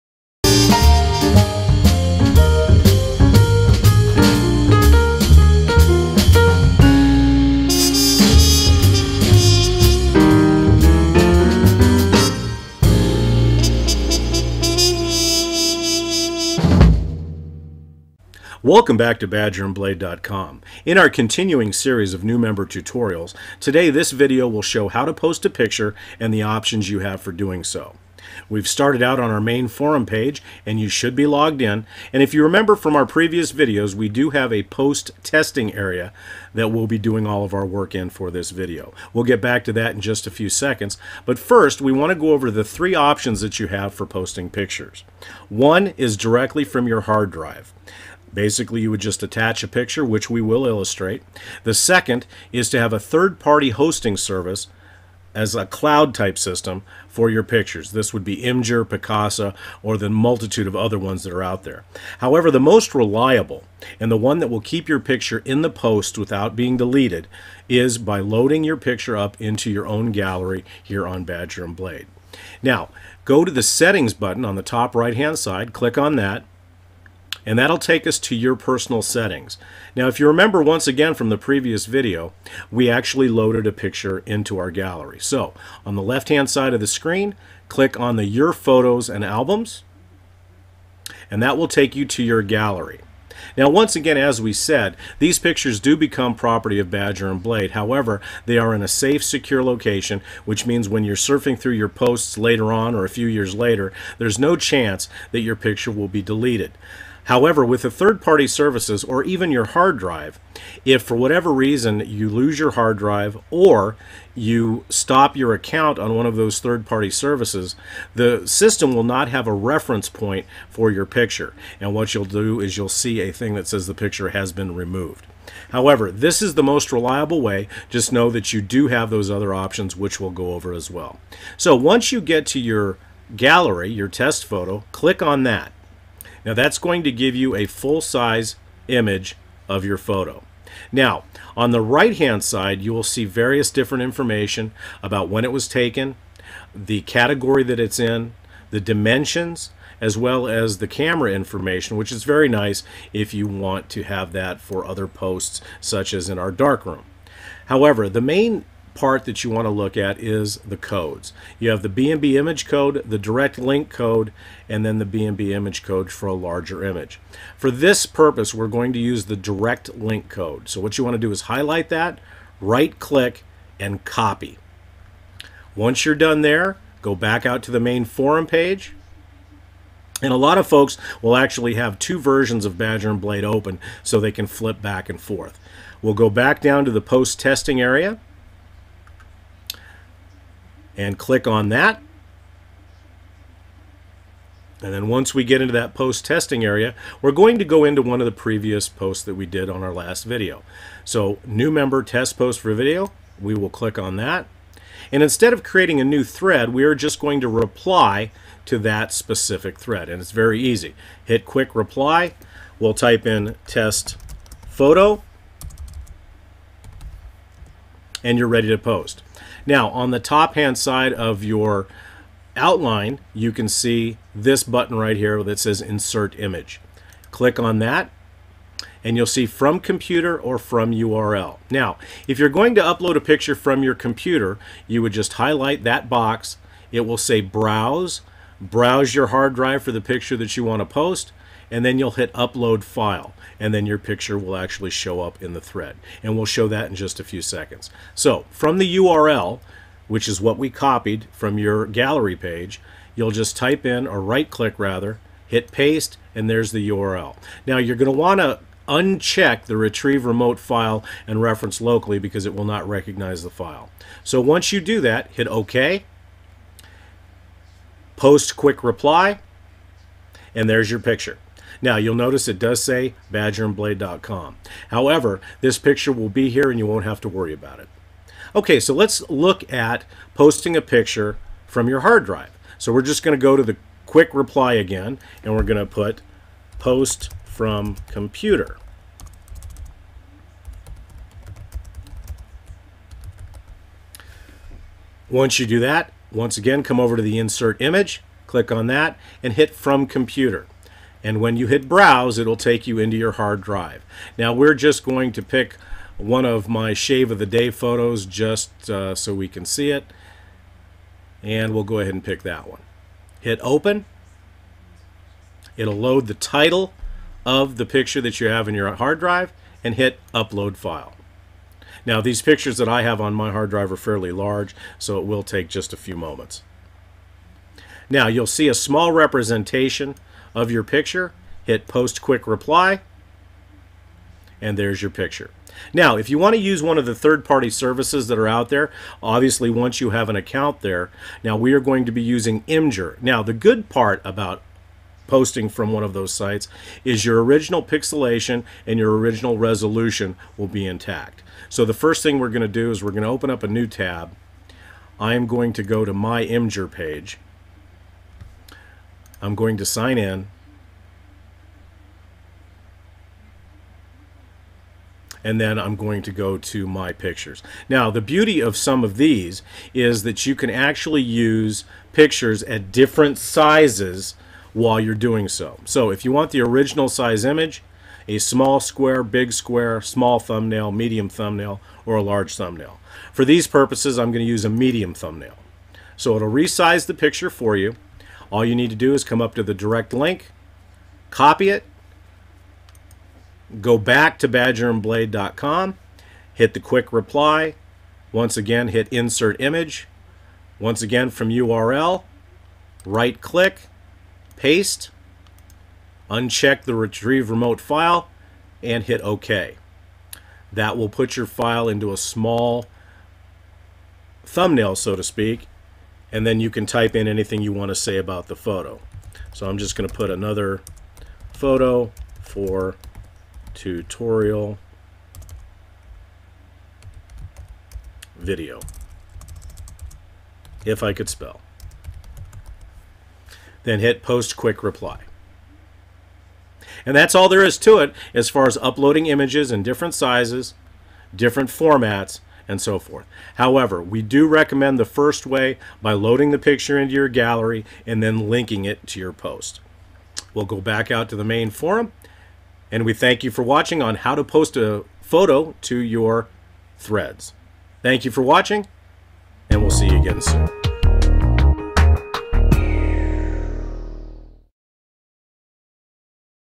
B & B How to post Photos and options
Badger & Blade's tutorial on how to post photos and photo options in your threads and posts